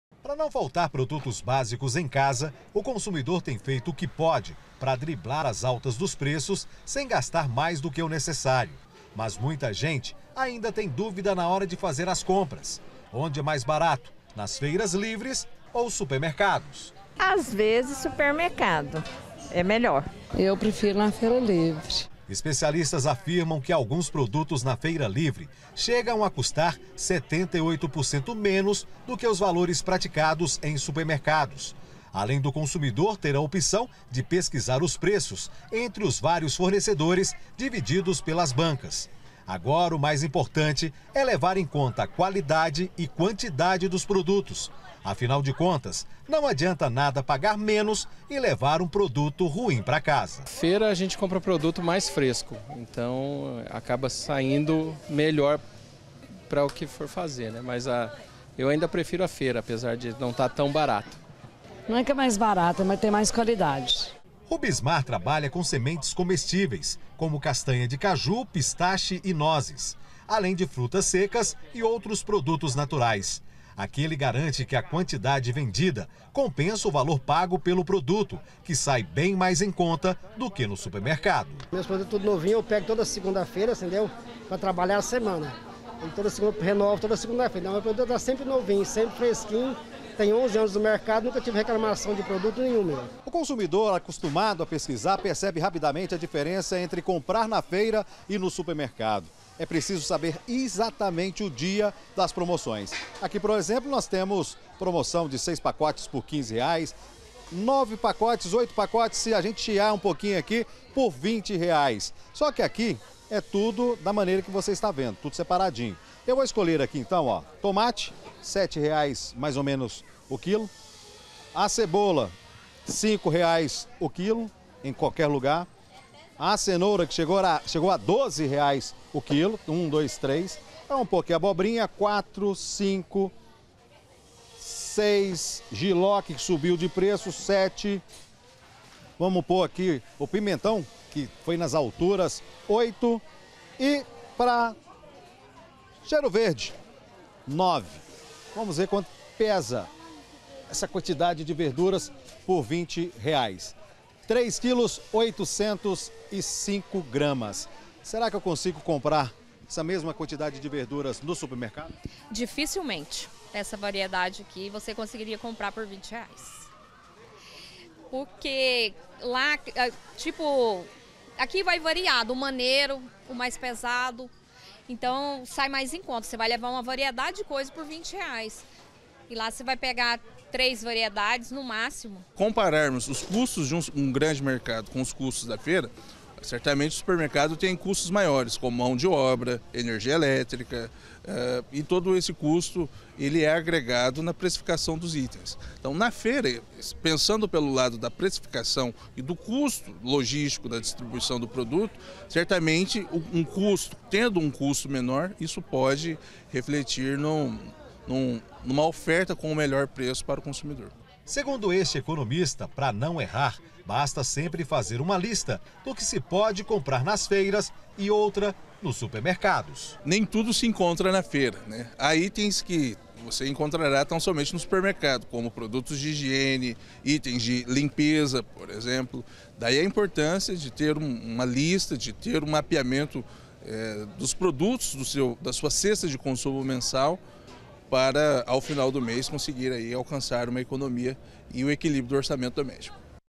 DILEMA DO BOLSO: FEIRA OU MERCADO, QUAL TEM O MELHOR PREÇO?
FAZER A COMPRA DO MÊS TÁ CADA VEZ MAIS CARO. COM ALTA NO PREÇO DOS ALIMENTOS, A OPÇÃO É PROCURAR LOCAIS COM MELHOR VALOR E VARIEDADE. SE TIVER PROMOÇÃO, AJUDA MUITO. AÍ FICA A DÚVIDA, DE QUAL É LUGAR É MAIS BARATO, A FEIRA OU MERCADO?. QUEM TIRA ESSA DÚVIDA PRA GENTE É O REPÓRTER DOUGLAS FERNANDES.